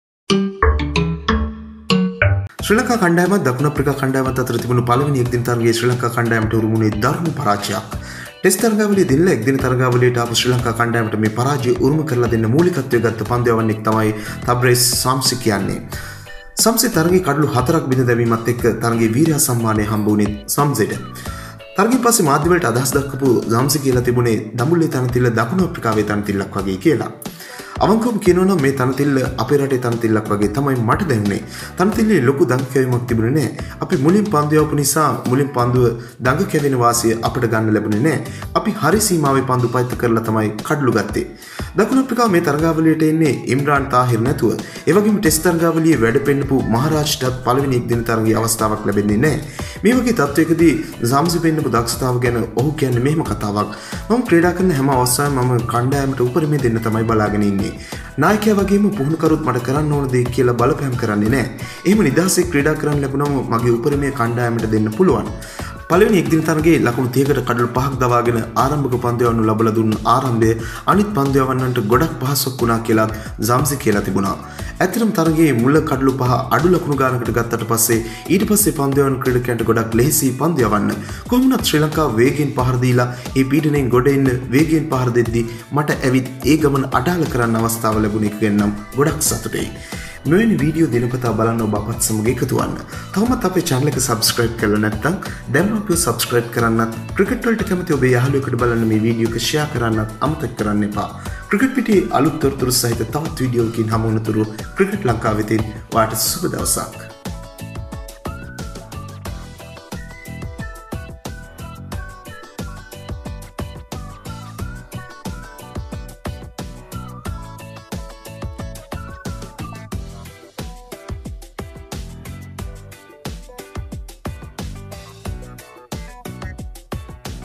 දඹුල්ල ගෙදර වගේදකුණු අප්‍රිකා පන්දු යවන්නාගෙන් අපුරු කථාවක්
ශ්‍රී ලංකා කණ්ඩායමත් දකුණු අප්‍රිකා කණ්ඩායමත් අතරේ තිබුන පලවෙනි එක්දින තරඟයේ ශ්‍රී ලංකාවට උරුම වුනේ දරුණු පරාජයක්,මේ පරාජය උරුම කරලා දෙන්න ලොකු දායකත්වයක් දුන්න පන්දු යවන්නෙක් තමයි දඟ පන්දු යවන ටබේෂ් ශම්සි..